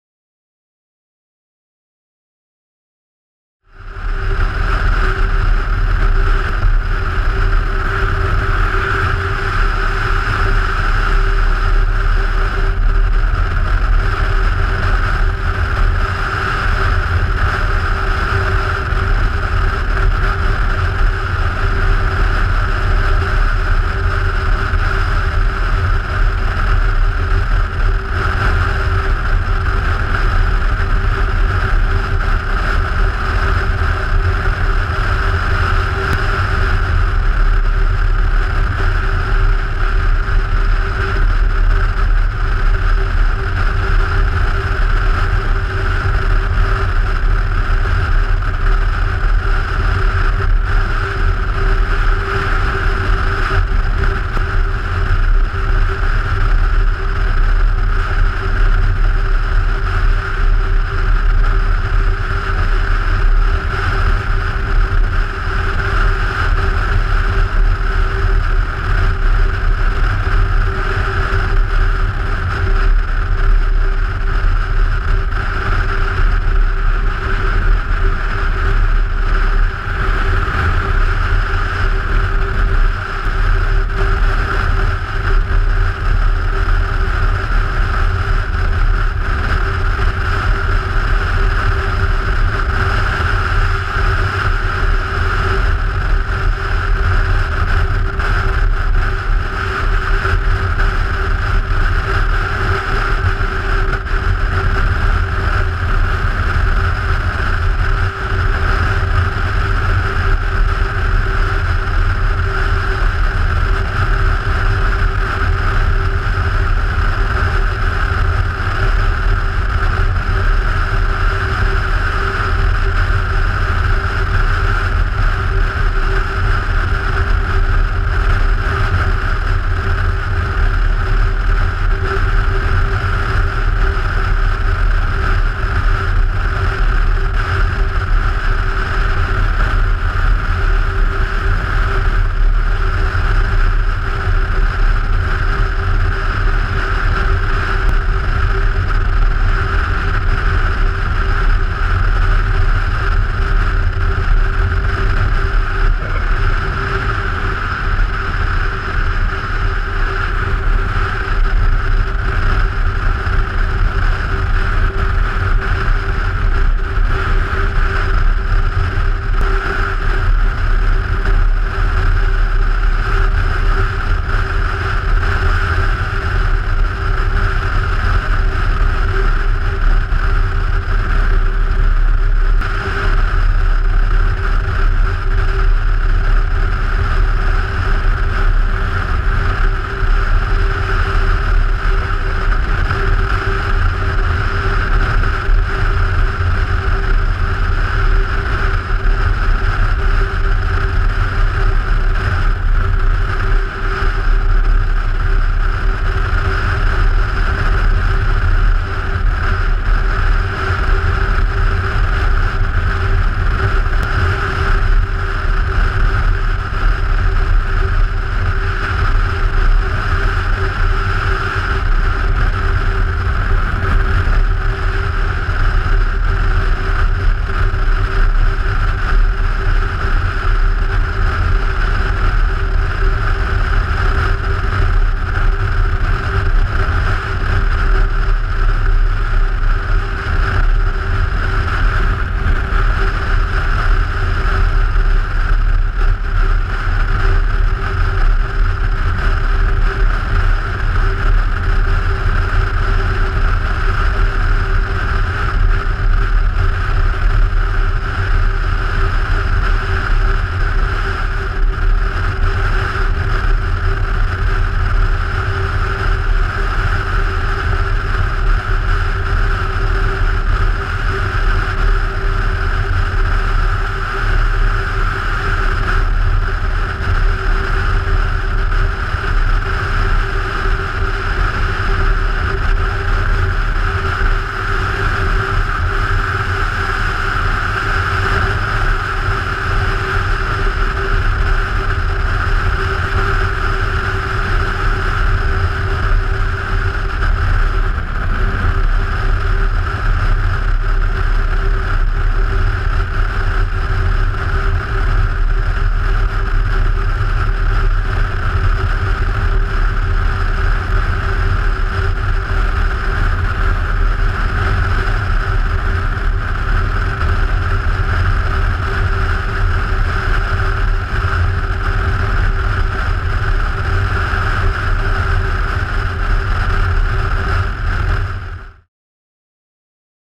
Femundsmarka 2015 ... Rv 26 "Ho dalen"
Rv 26 from Tolga to our camp at Langsøen